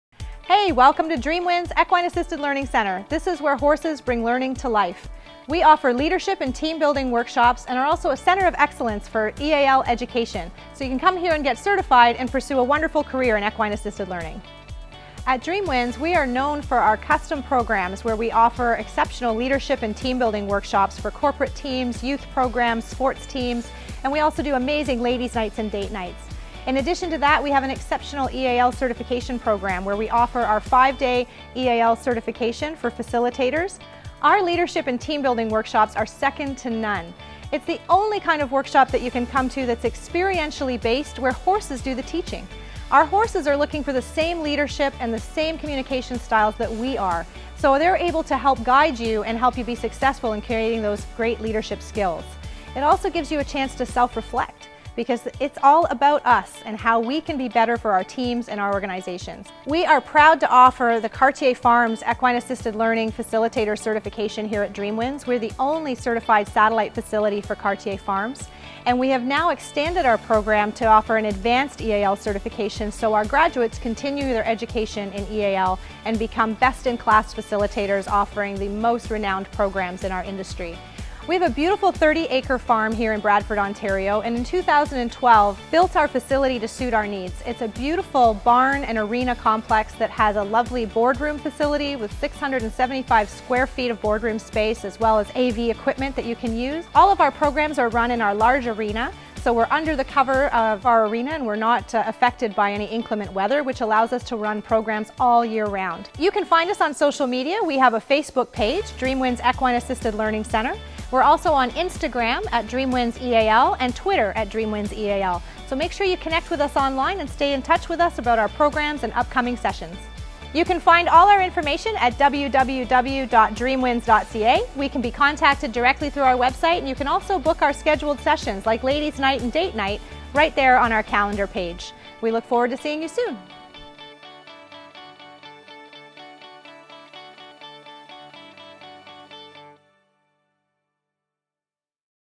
An Introduction to Dreamwinds EAL
There are so many applications of Equine Assisted Learning.  At Dreamwinds we offer Team Building and Leadership Training, Youth Leadership Development, Ladies Night and Date Night as well as provide an EAL career path with our Certified EAL Facilitator Training.